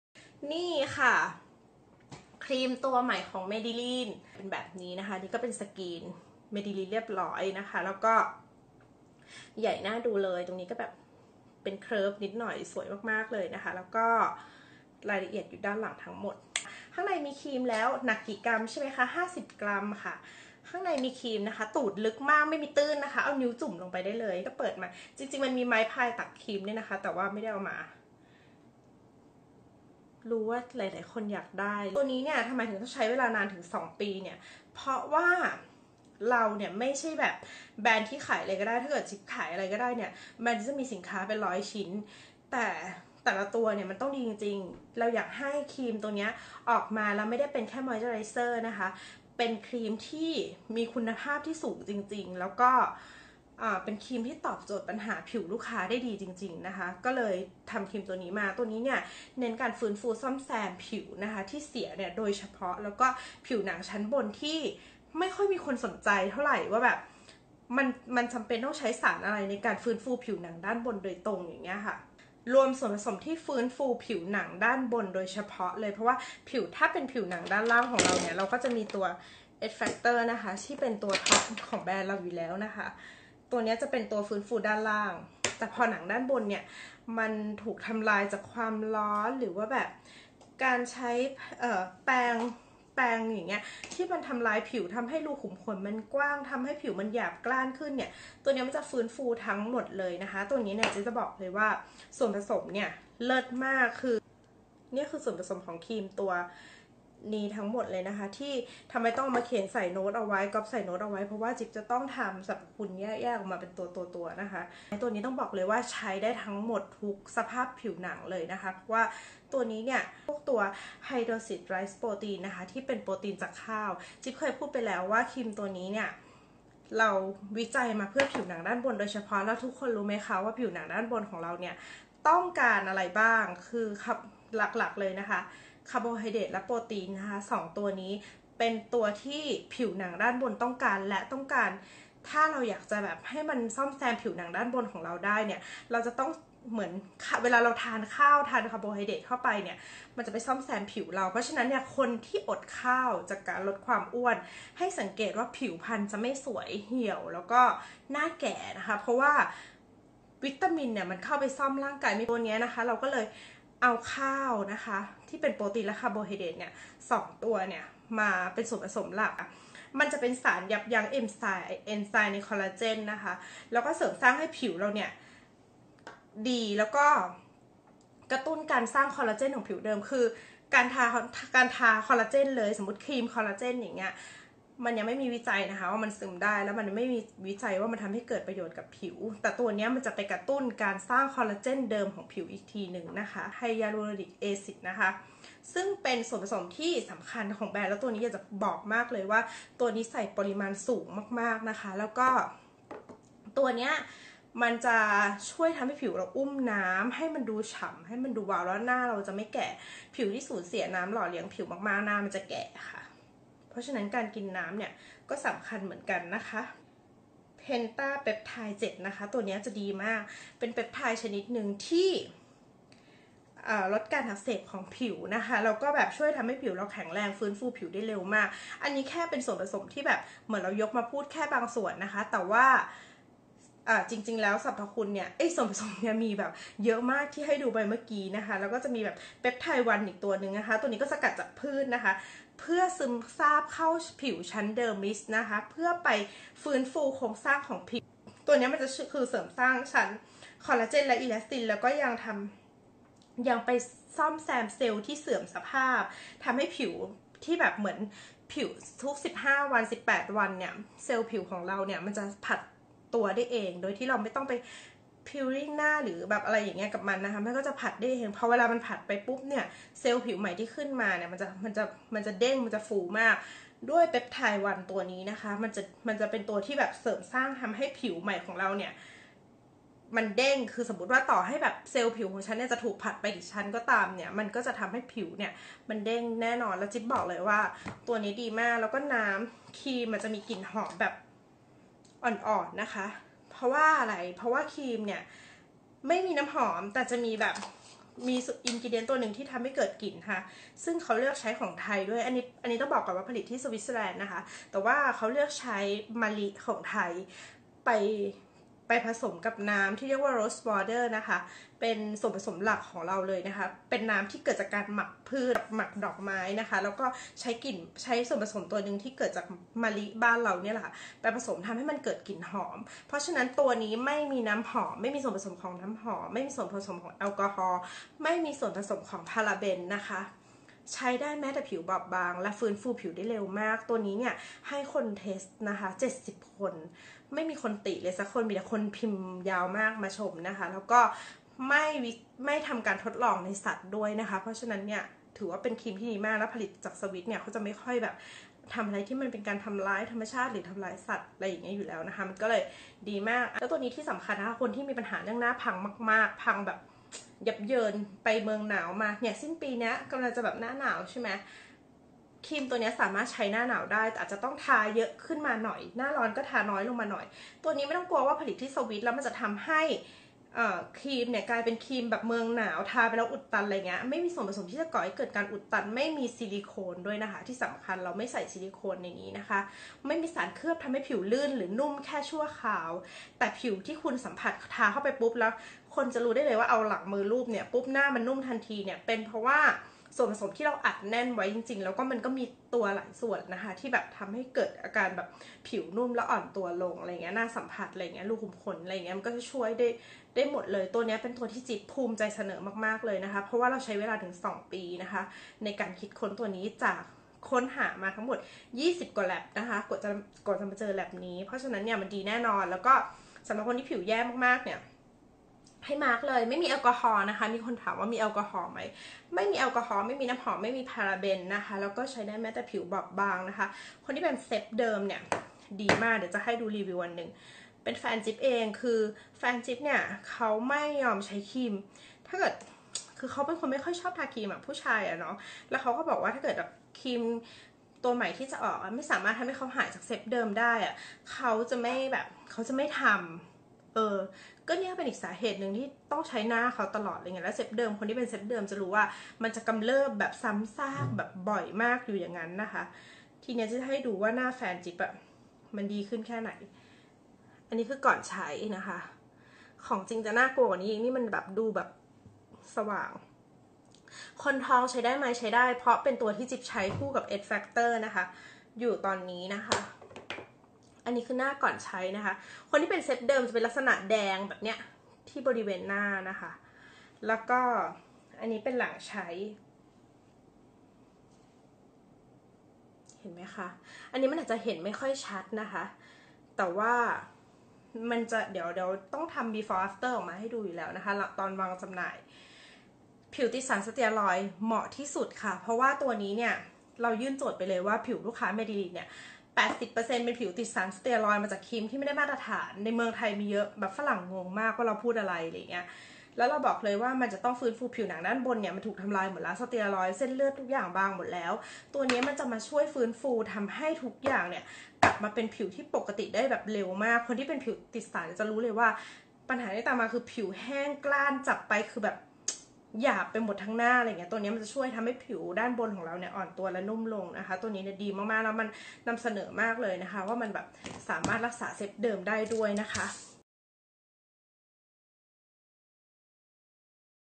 ครีมหน้าเด็ก - Medileen Advanced Rejuvenating Cream
Medileen Advanced Rejuvenating Cream

• ครีมตัวนี้ผลิตจากแล็บที่มีความเชี่ยวชาญโดยเฉพาะที่ประเทศสวิสเซอร์แลนด์ ที่ผลิตให้กับเคาน์เตอร์แบรนด์ดังยี่ห้อหนึ่งระดับsuper hi end  แต่มีคุณหมอที่ปรึกษาแบรนด์เมดิลีนคอยคัดสรรส่วนผสมที่คิดค้นมาสำหรับผิวคนไทยโดยเฉพาะ

• ช่วยรักษาและฟื้นฟูผิวติดสารสเตียรอย  บรรเทาอาการเซบเดิร์ม ช่วยรักษาเรื่องริ้วรอย เมื่อใช้ต่อเนื่องเป็นระยะเวลานาน จะทำให้ผิวนุ่มและเปล่งประกายราวผิวเด็ก

• สามารถใช้เป็น sleeping mask ได้ด้วยนะคะ หากต้องการฟื้นฟูผิวอย่างเร่งด่วน โดยการทาหนาๆก่อนนอน ตื่นขึ้นมาผิวจะนุ่มมม ใส เด้ง ราวกับผิวเด็ก

💧 ฟื้นฟูผิวหนังชั้นบนอย่างเร่งด่วน
💧 กักเก็บน้ำหล่อเลี้ยงผิว
💧 เติมเต็มริ้วรอย ร่องลึก
💧 กระตุ้นการสร้างคอลลาเจน
💧 ช่วยให้ผิวเรียบเนียนกระชับ
💧 เติมความชุ่มชื้น
💧 ช่วยให้ผิวกระจ่างใส
💧 บล็อคการก่อตัวของเม็ดสีเมลานิน
💧 เพิ่มความอ่อนเยาว์ให้ผิว
💧 ผิวสวยเปล่งประกายราวผิวเด็ก

• 🇨🇭Made in Switzerland🇨🇭
ปริมาณ 50g. ราคา 1850฿ ส่งฟรีems